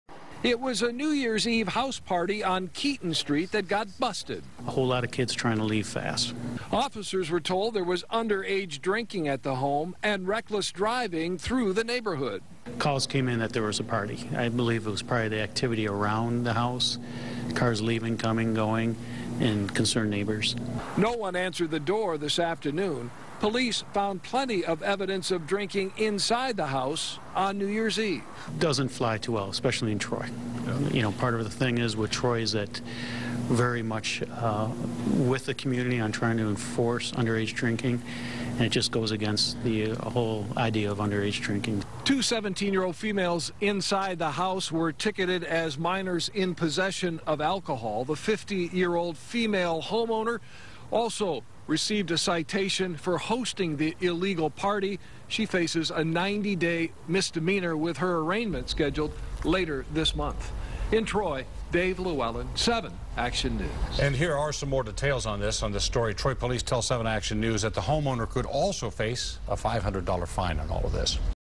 Mom facing fine over party
A Troy mother is facing a fine over underage drinking at her home during a New Year's Eve party.